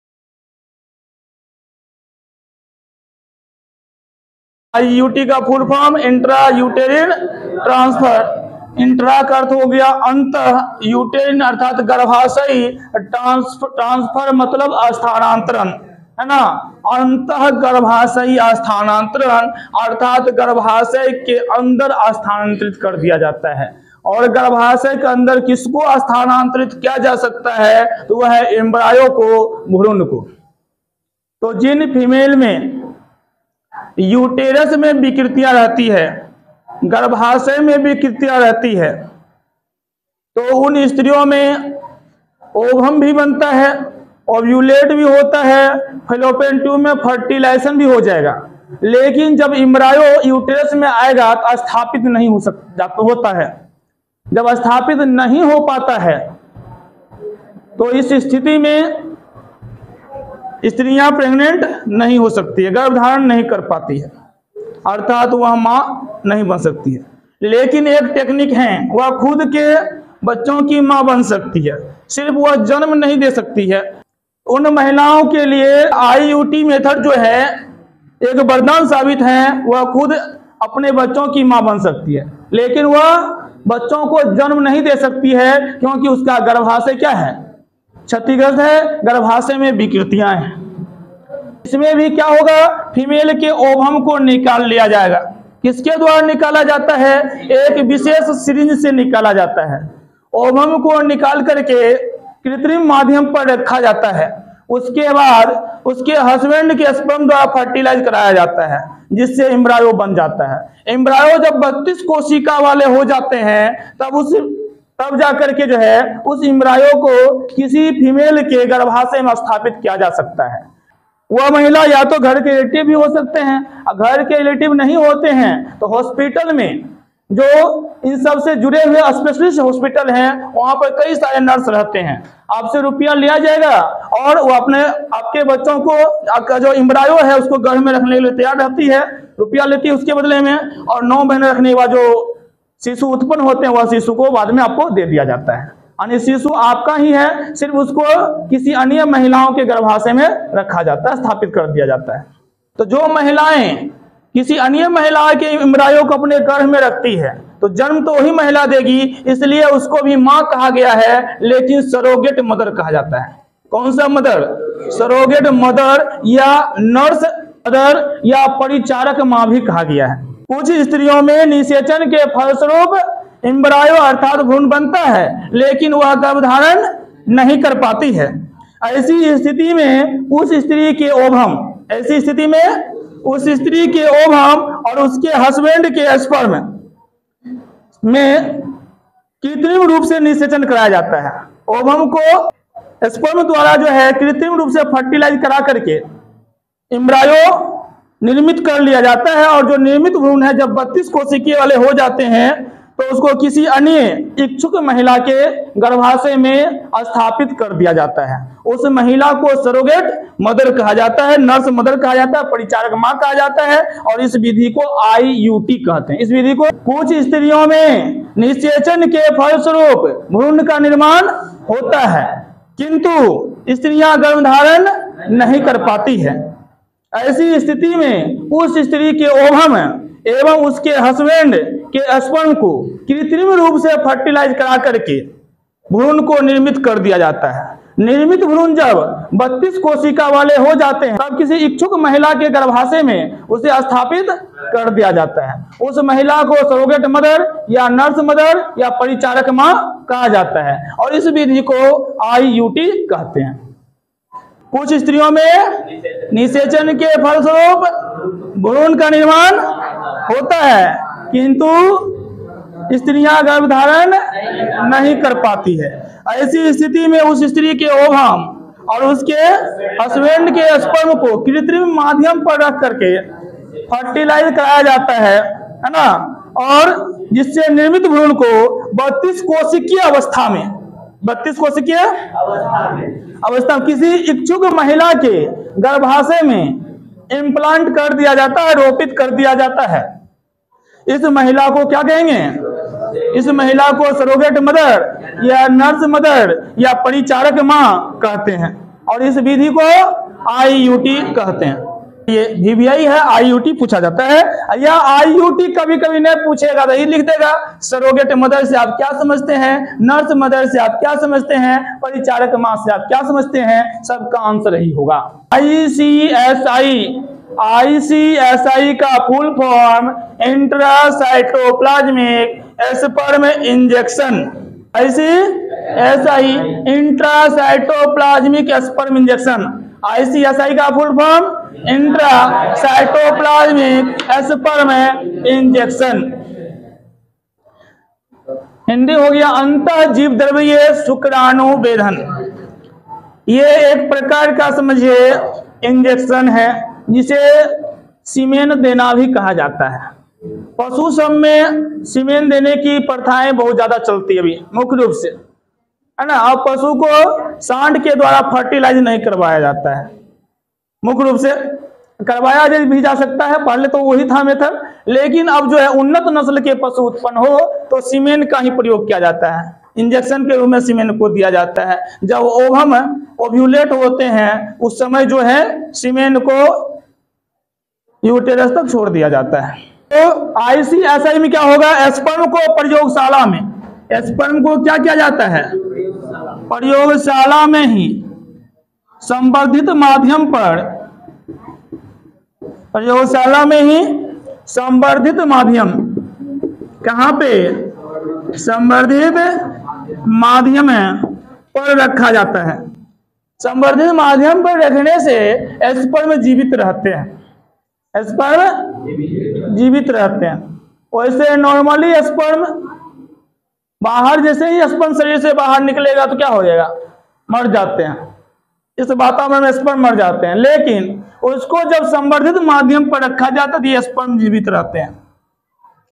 IUT:सरोगेट मदर या नर्स मदर ,ICSI#class12 biology lesson 04 जनन स्वास्थ्य#by :om sir
सरोगेट मदर बंध्य स्त्री के लिए वरदान साबित हुई है ।